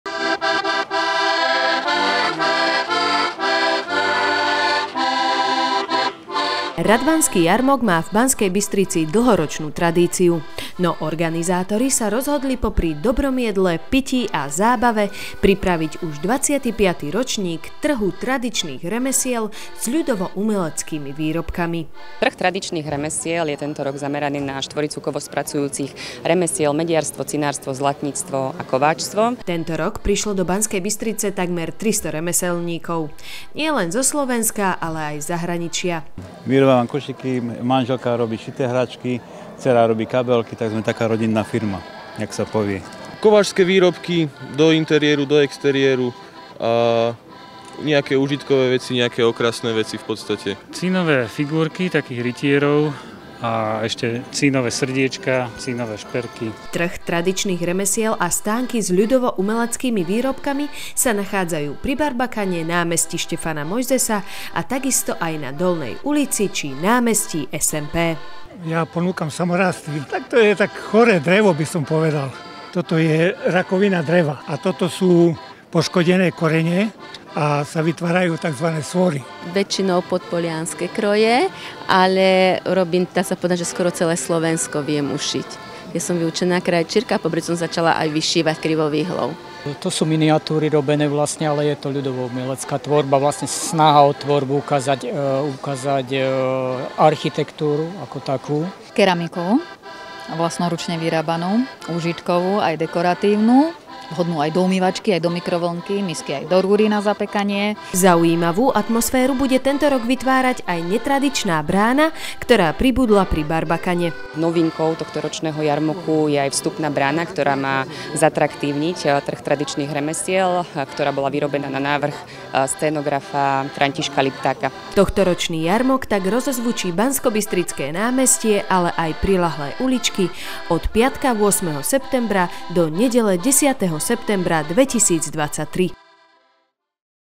Radvanský jarmok a trh tradičných remesiel 2023
V Banskej Bystrici sa po 365 - krát koná tradičný Radvanský jarmok. Mesto sa tak od 7. do 10. spetrembra opäť stalo dejiskom prezentácie tradícii, zvykov ale aj zábavy a dobrého jedla.